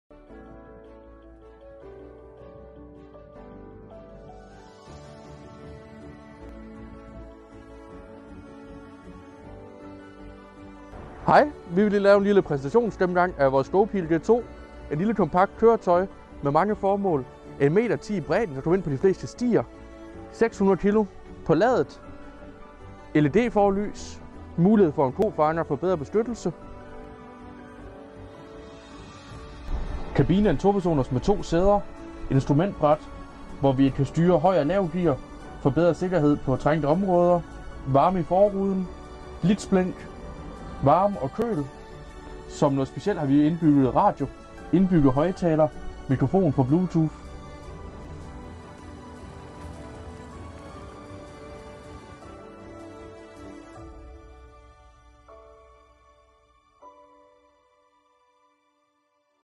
Goupil elbil - prøv den GRATIS, kontakt Anker Bjerre A/S
Tænker du på:
- miljøet og klimaet?
- arbejdsmiljø og nærmiljø?
Så er Goupil elbilerne måske lige noget for dig!

Kontakt Oliver Povlsgaard fra Anker Bjerre A/S på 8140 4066 og lav en aftale allerede i dag.